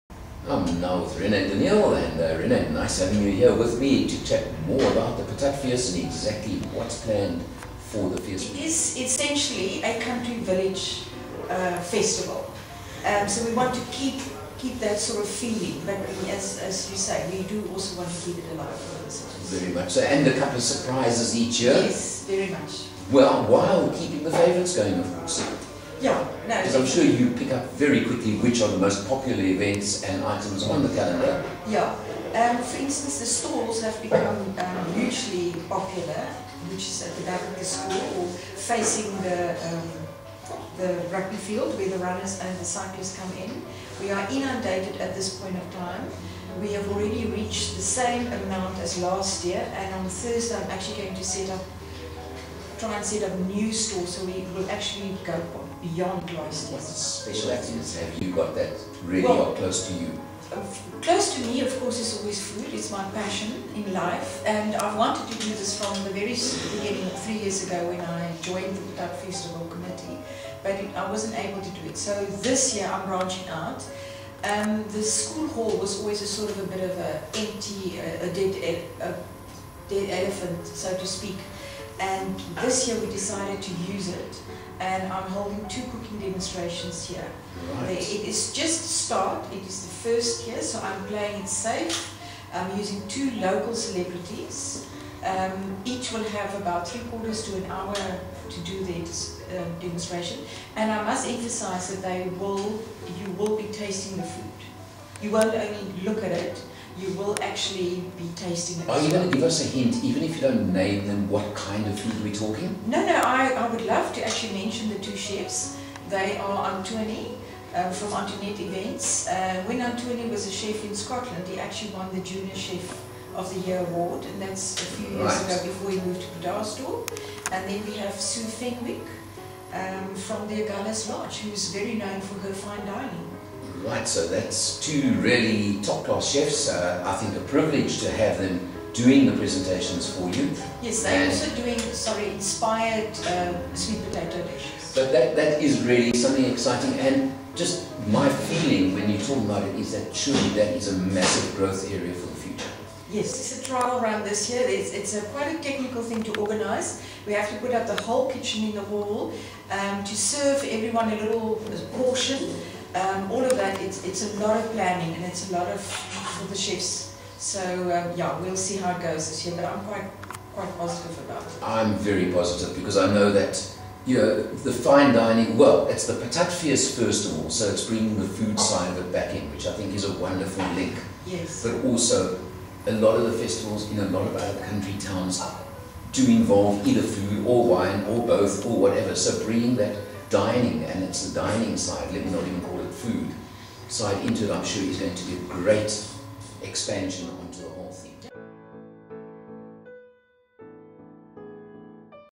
Napier Patatfees (the Renee Daneel interview).mp4
A Professionally laid-out kitchen with PROFESSIONAL CHEFS giving cooking demonstrations WITH TASTINGS ... listen to RENEE DANEEL telling Alan about what to expect this weekend